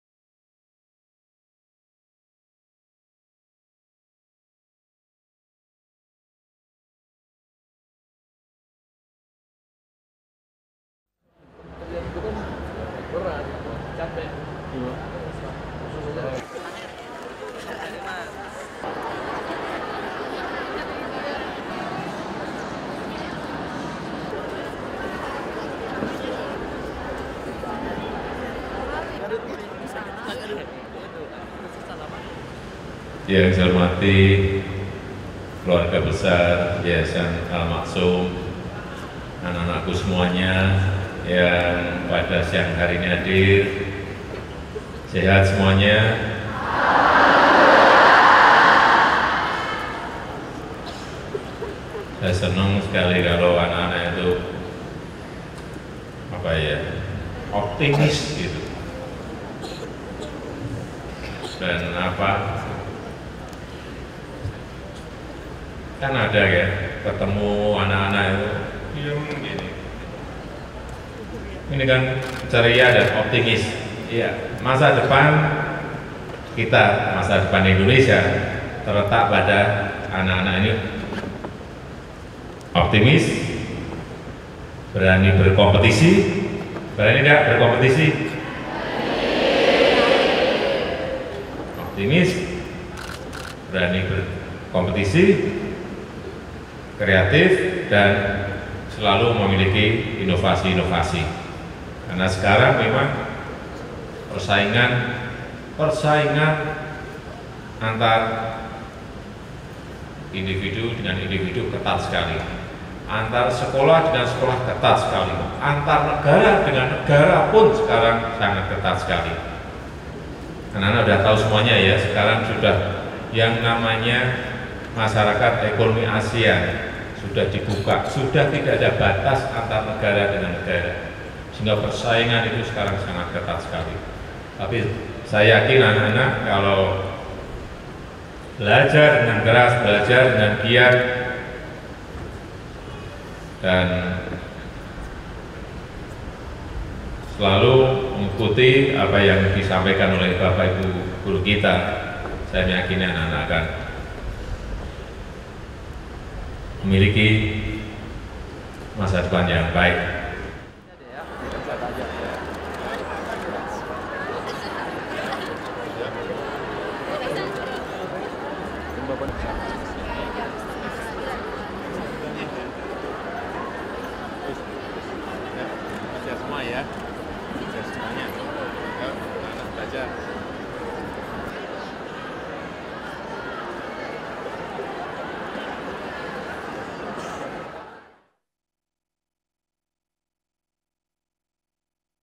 Senangnya Siswa SMP-SMA Yayasan Al Ma'soem Dikunjungi Presiden Jokowi
Siswa SMP-SMA Yayasan Al Ma'soem Sumedang Dikunjungi Presiden Jokowi, 8 Agustus 2016. (Sumber: Biro Pers Istana)